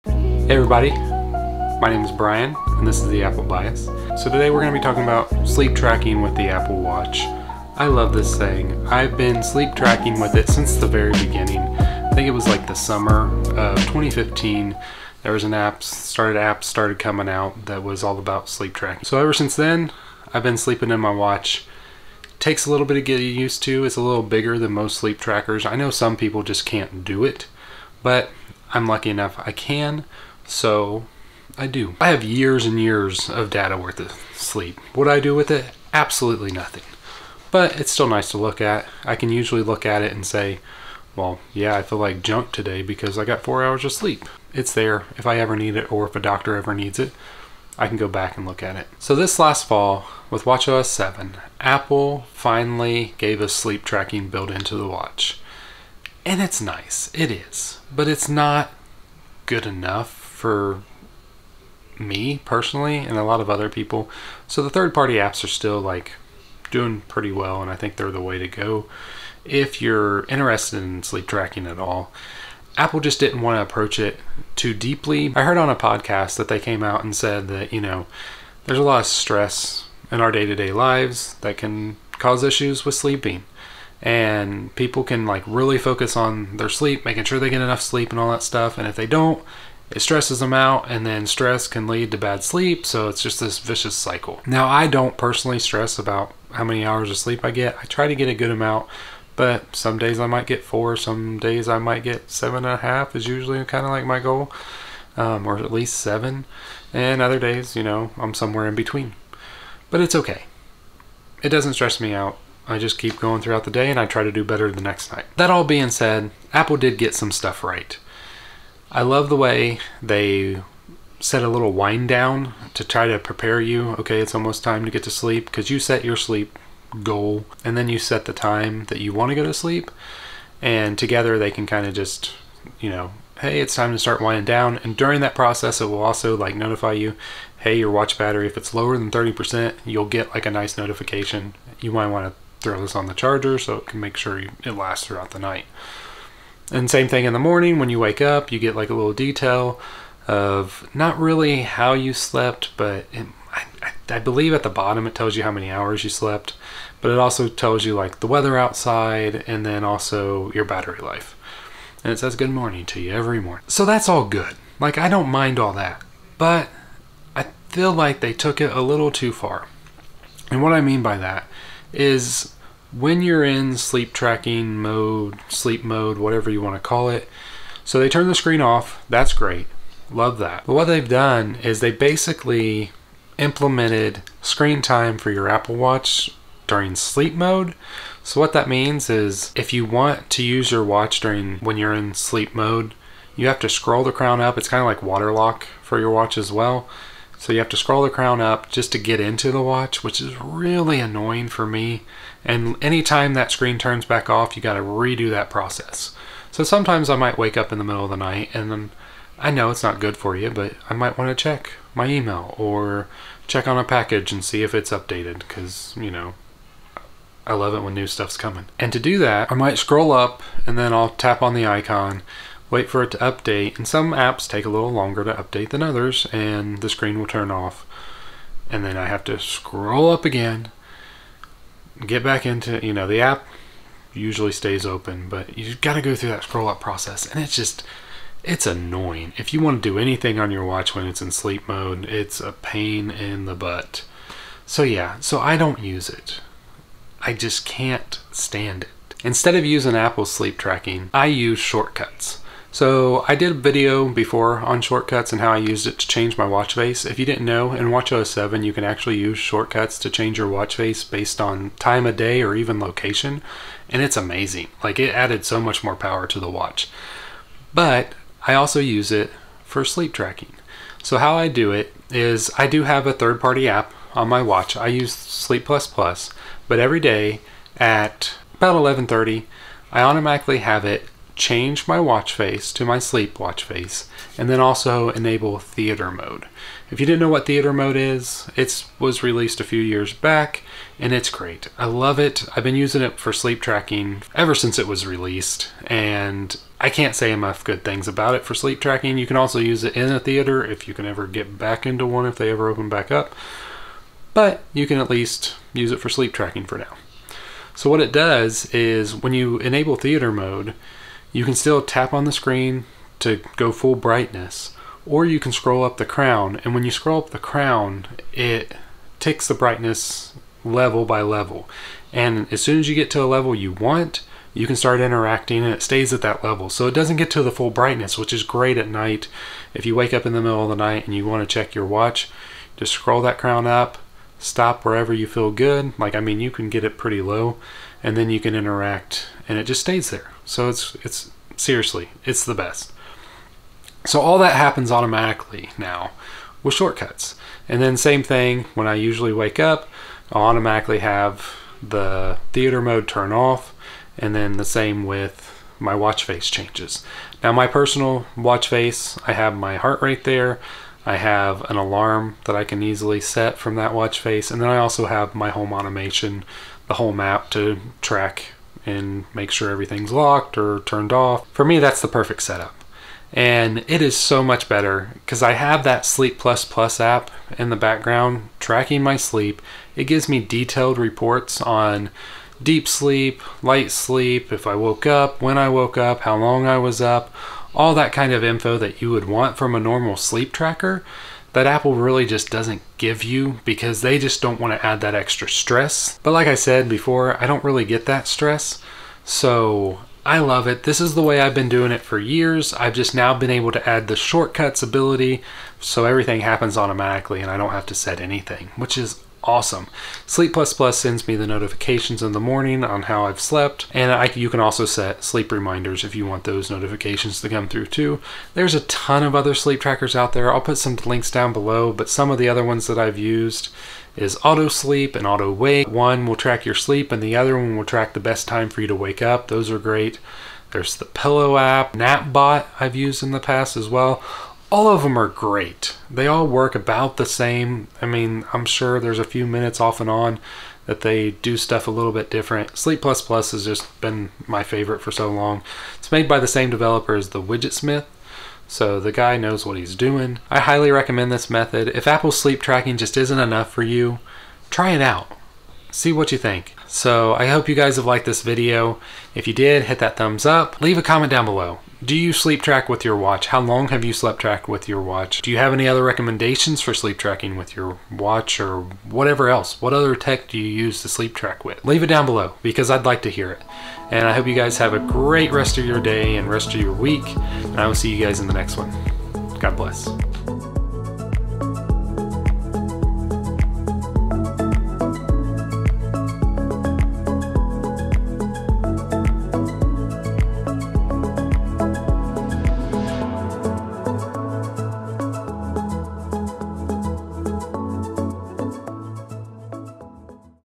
The Best Way to Sleep with the Apple Watch - Don't use Apple's Method
Apple built in sleep tracking to watchOS 7, but they took more of a hands off approach. I used it for a few months, but gave up cause it was just lacking too much info and added more annoyances then benefits.

I highly recommend just using Apples shortcuts. The Shortcuts feature that allows you to change you watch face at certain times of the day or based on locations. I use that with also setting theater mode that turns off the screen and allows the user to set the brightness based on the scrolling up the dial. I then use Sleep++ to track my actual sleep it give me a more detail look at how well I actually slept.

My other video on using shortcuts to change the watch face automatically on the watch: